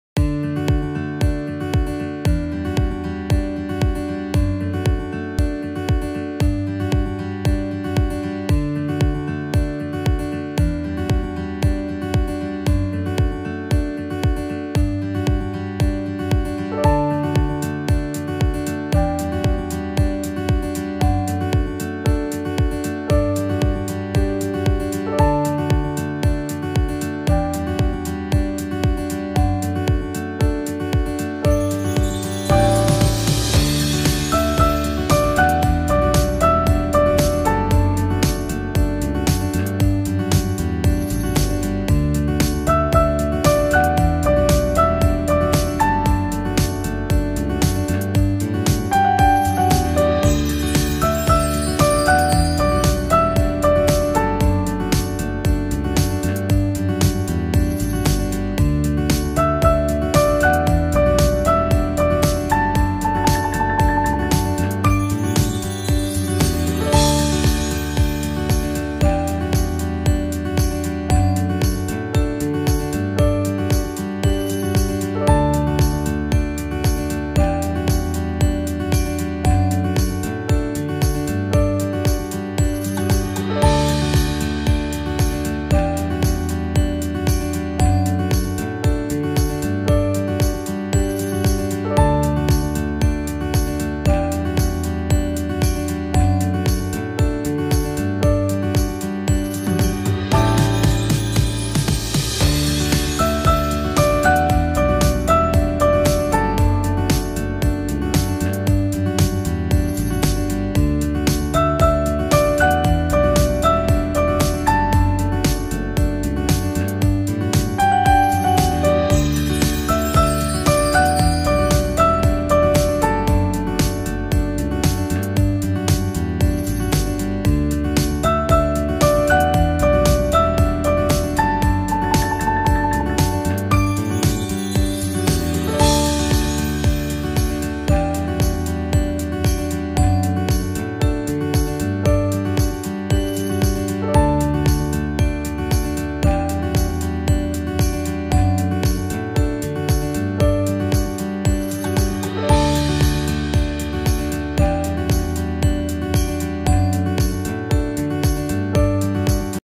Islamabad City covered in Fogاسلام آباد دھند کی لپیٹ میں ہے، بلند و بالا عمارتیں اور اوپر سے دھند
Islamabad City covered in Fog

اسلام آباد دھند کی لپیٹ میں ہے، بلند و بالا عمارتیں اور اوپر سے دھند کا منظر
कोहरे में ढका इस्लामाबाद, ऊँची-ऊँची इमारतें और कोहरे के ऊपर से नज़ारा